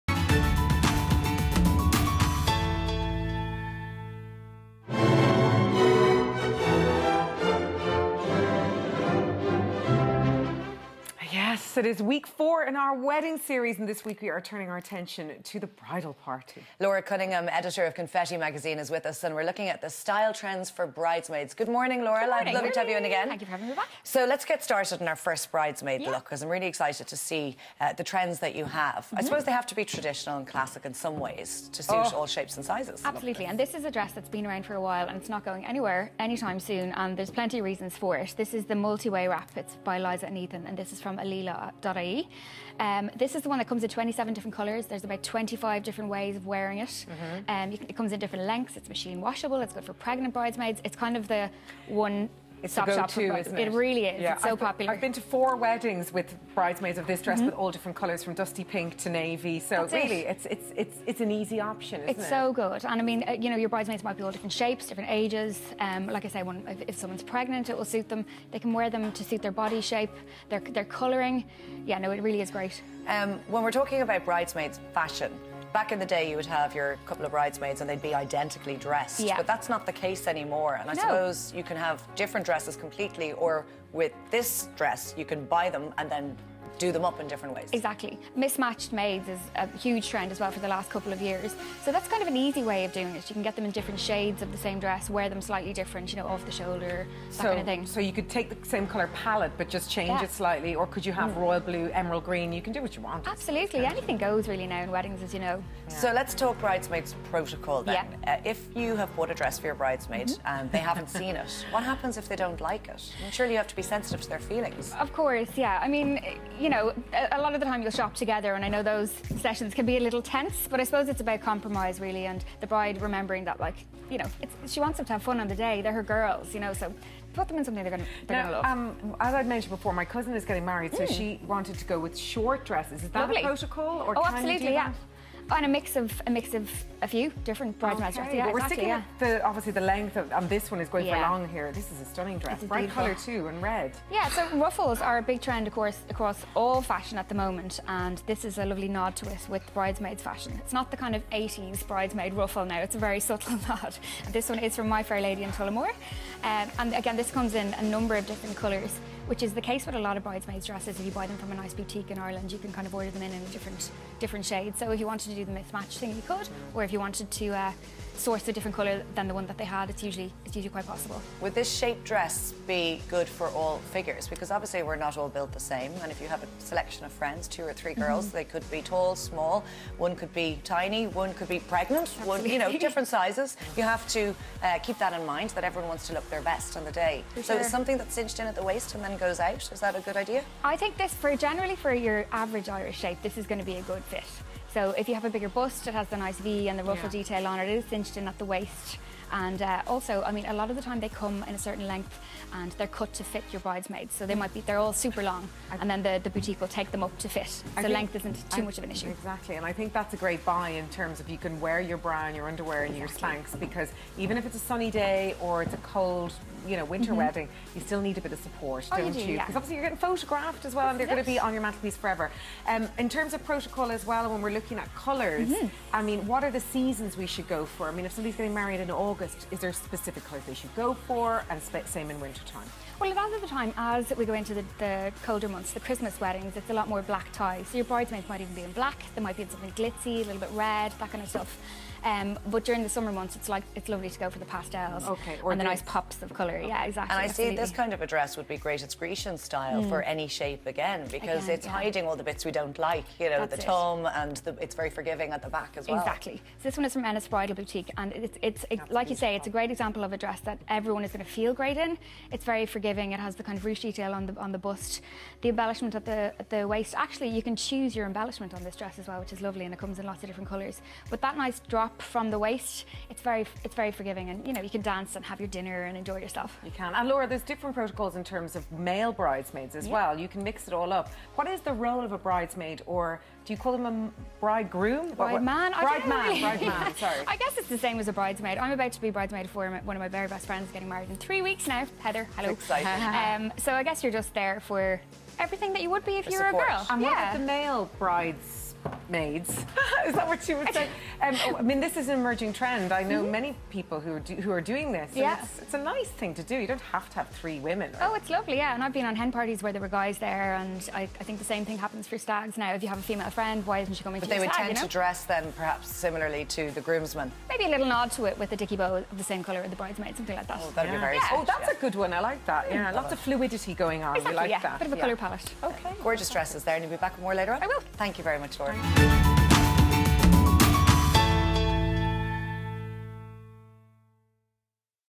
Bridesmaid Fashion Part 1
It's week four in our Wedding Series and this week we're turning our attention to the bridal party.Laura Cunningham, editor of Confetti Magazine is with us and we're looking at style trends for bridesmaids.